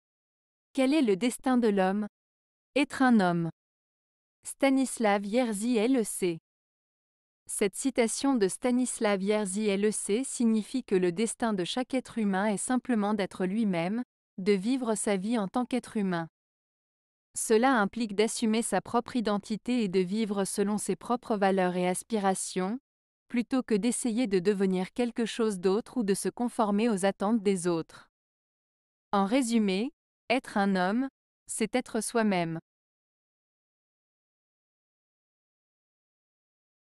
Quel est le destin de l'homme ? Etre un homme. - Stanislaw Jerzy Lec
Cette citation de Stanislaw Jerzy Lec signifie que le destin de chaque être humain est simplement d'être lui-même, de vivre sa vie en tant qu'être humain. Cela implique d'assumer sa propre identité et de vivre selon ses propres valeurs et aspirations, plutôt que d'essayer de devenir quelque chose d'autre ou de se conformer aux attentes des autres. En résumé, être un homme, c’est être soi-même.